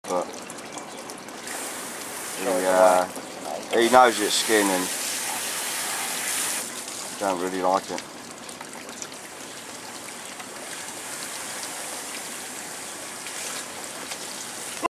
Tarpon dont like skin
Jeffs pet tarpon makes a visit every time he cleans fish, but will it eat dolphin fish skin?